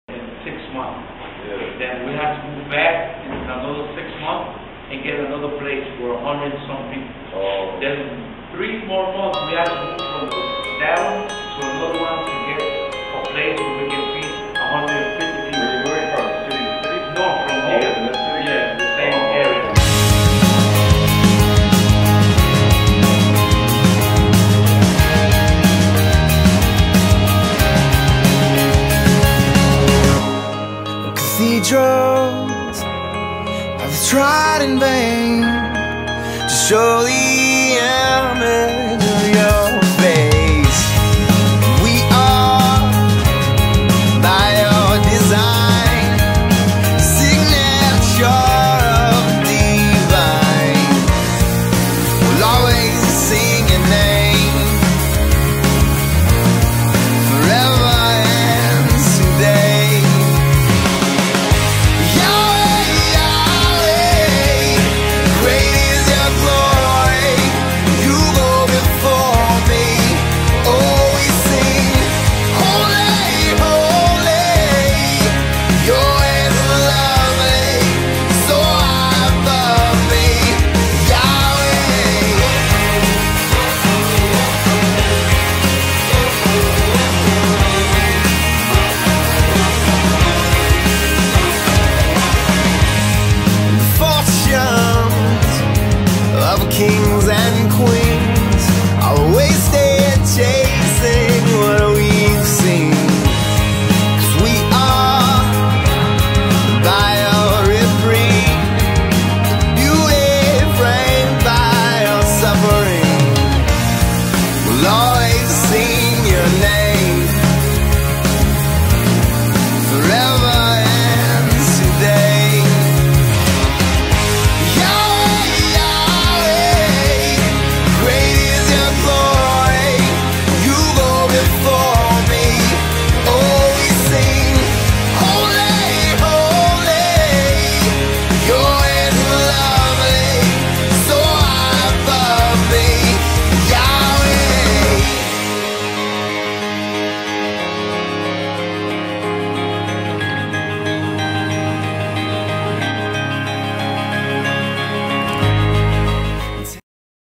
Dominican Republic Missions Trip July 2008 The Cause Community
July 2008 Mission Trip to the Dominican Republic from The Cause Community Church.
Pete and John Garra, Stephanie Ausensi, Hugo Martinez, Terryl, Margie Shimmel, Ryan and Desiree Garcia, Danielle Esquivel